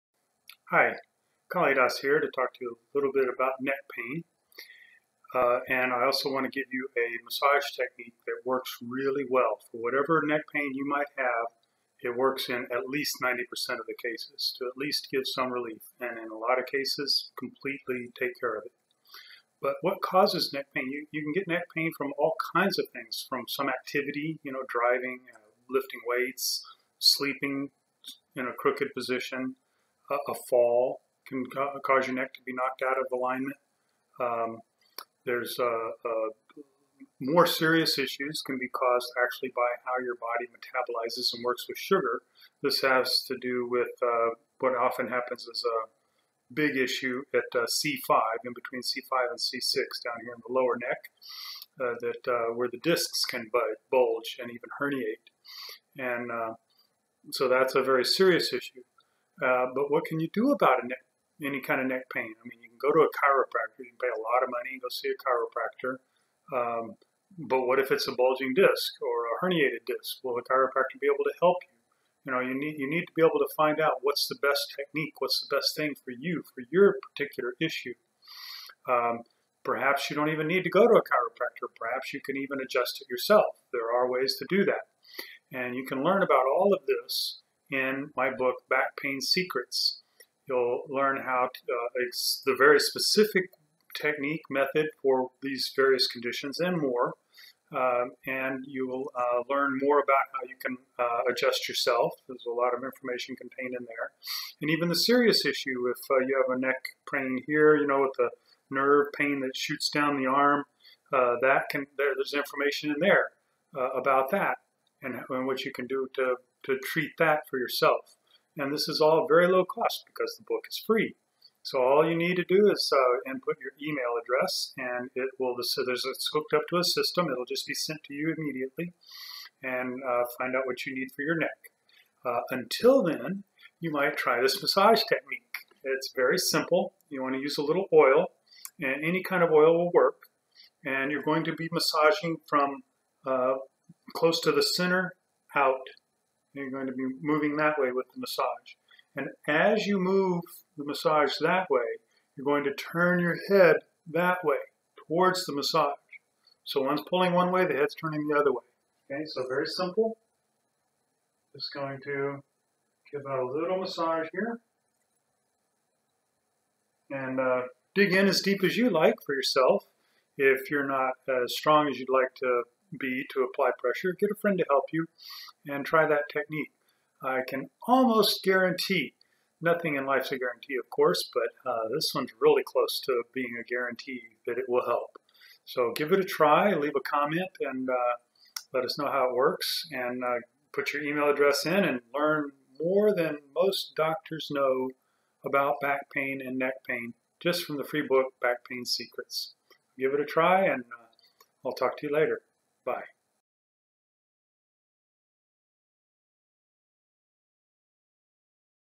Free Book Neck Pain Relief
Get your free copy today and you will be on your way to freedom from neck pain fast. Back Pain Secrets contains the best possible treatment for your particular neck issue. It also has information on how you can learn to adjust your neck yourself for immediate pain relief.

Back Pain Secrets has the information you need to get the right treatment for your particular back pain. Holistic techniques work best in alleviating back pain as has been proven in multiple studies. So, get your copy today and end your pain the fastest way possible.

End your neck pain fast with Self Adjusting Technique that teaches how to adjust your neck, back, hips and ribs for immediate pain rerlief. One hundred percent guaranteed to work for you or your money back with no questions asked.